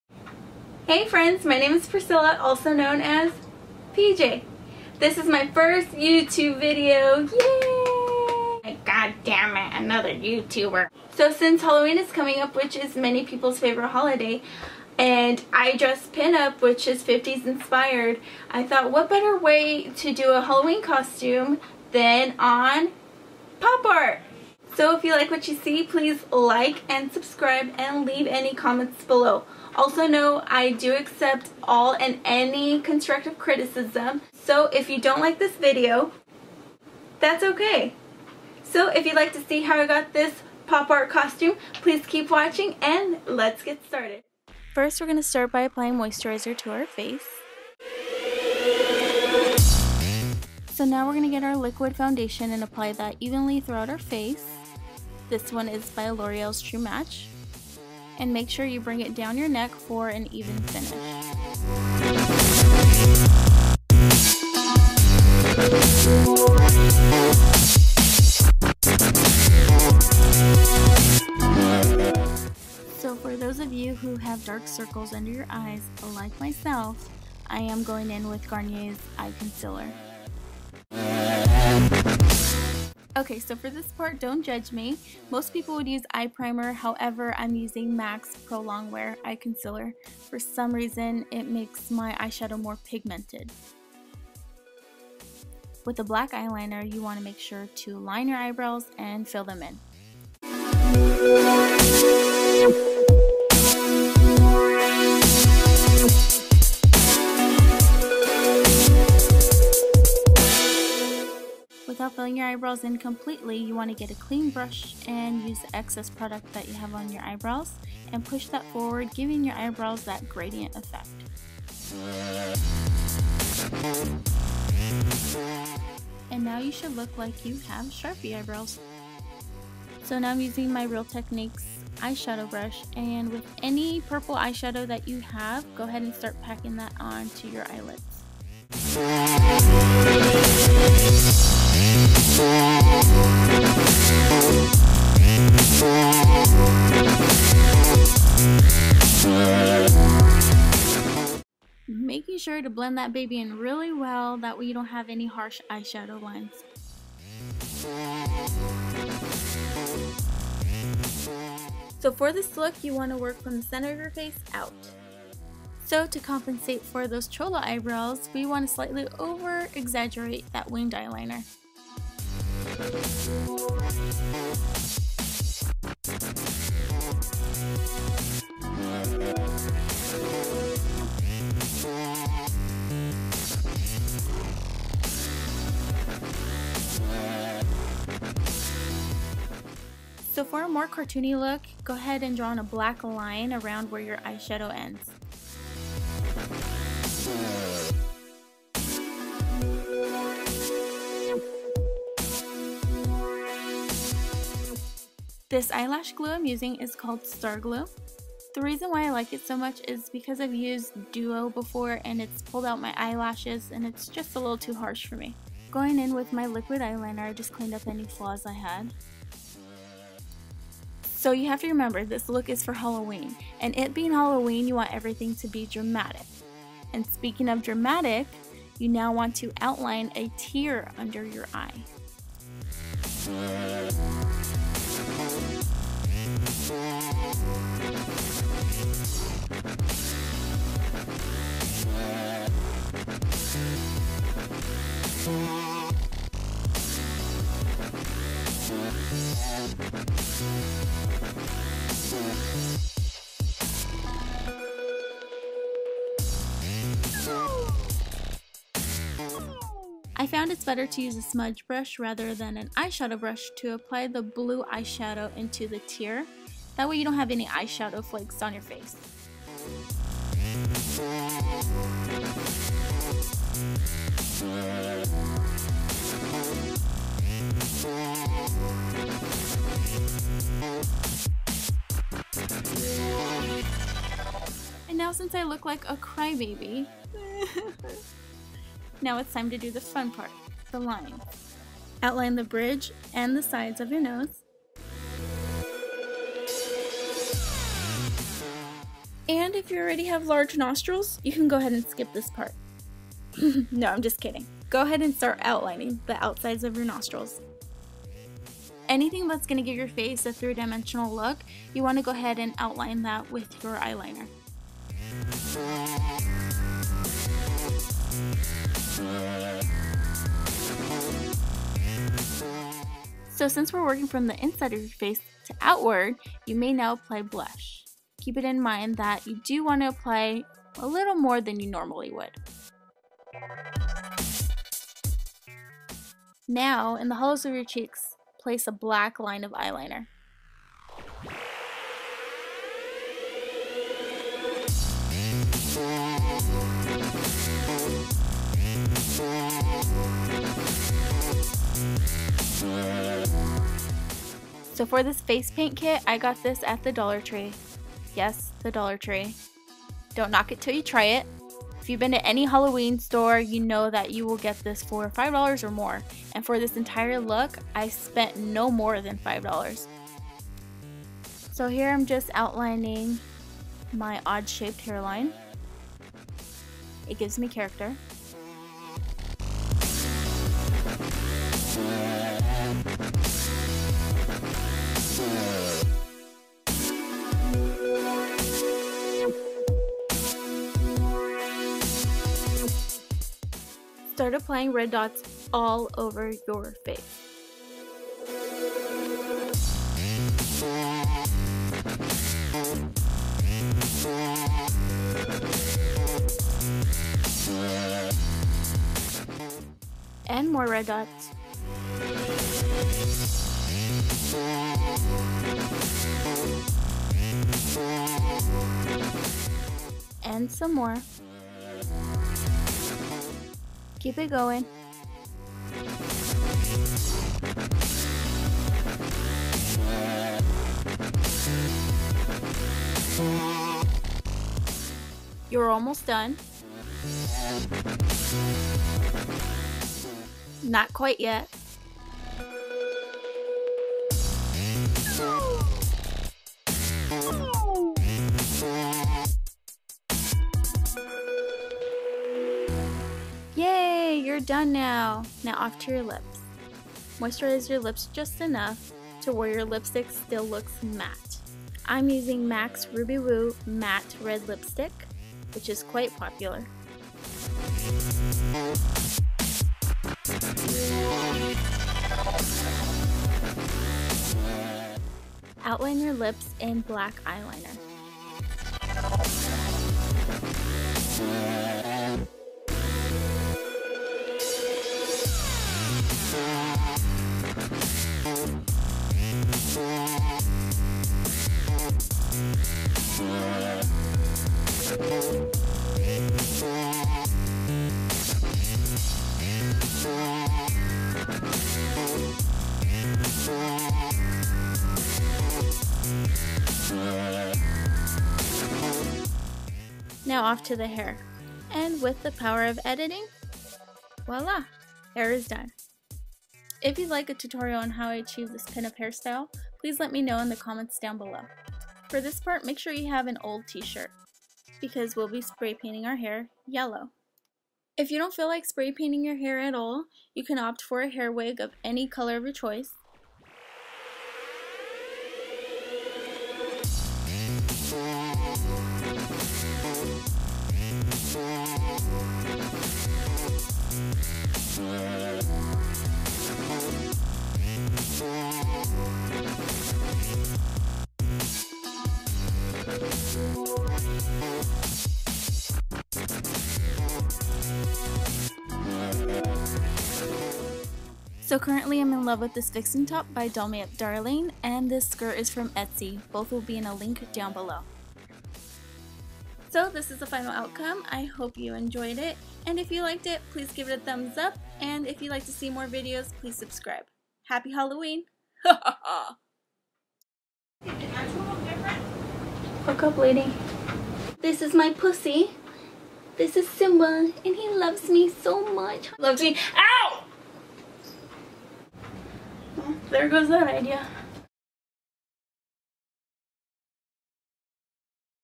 Pop Art / Comic Book Makeup Tutorial - Halloween Costume Idea
Pop Art Makeup Tutorial

Products used:
Face Paint Kit - Dollar Tree
Jerome Russell Yellow Hair Paint - Drug Store

I didn't show all my makeup products in this video because I think you can achieve this makeup look with any makeup you may have at home :) The main products that you need are list Above.

Face Makeup:
Moisturizer: Lumene BB Serum
Foundation: L'oreal True Match
Powder: MAC Studio Fix
Eye Concealer: Garnier Anti-Dark Circle
Eye Liner: Stiletto Maybelline
Mascara: Dior Addict by Dior
Eye Lash Glue: Star Glue
False Eye lashes: Sassi #73
Eye Shadow (Purple and Blue): HB Cosmetics
Blush: Nordstrom Palette
Lip Stick: Ruby Whoo by MAC

Camera:
Canon Rebel T5i DSLR with 18-55mm lens